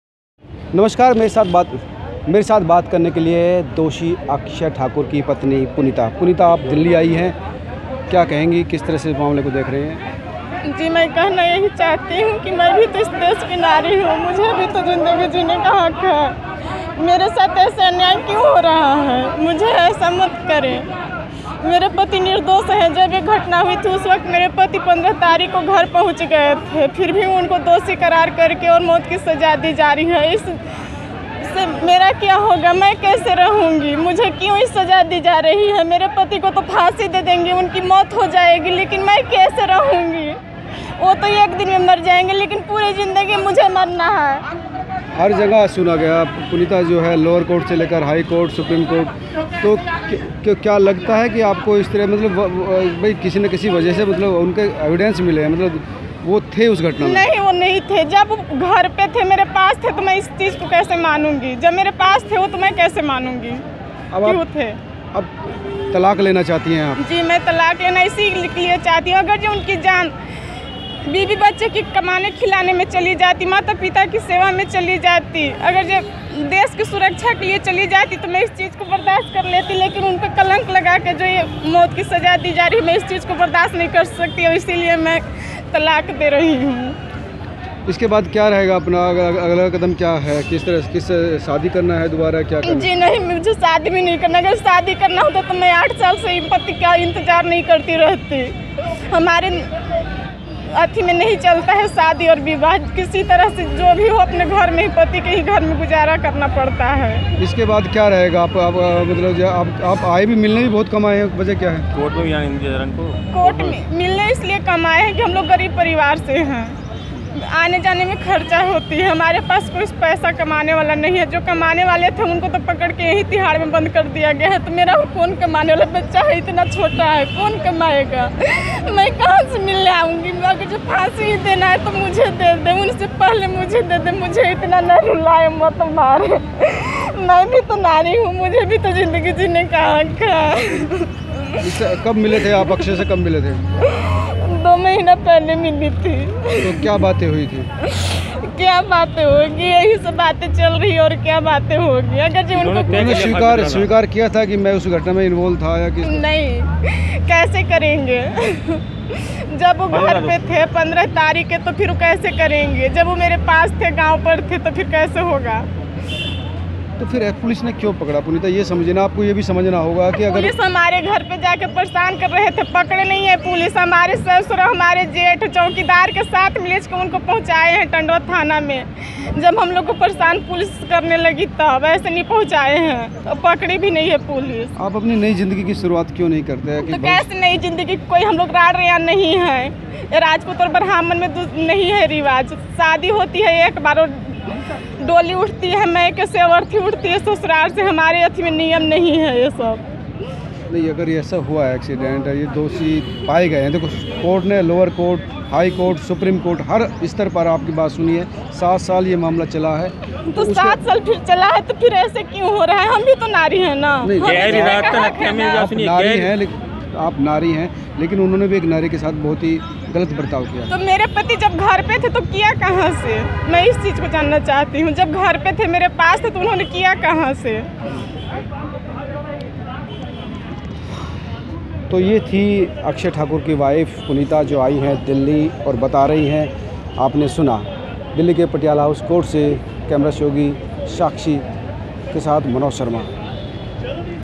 Akshaya thakur ke wife ka Exclusive Interview
Nirbhaya case Convicted akshaya thakur wife exclusive  interview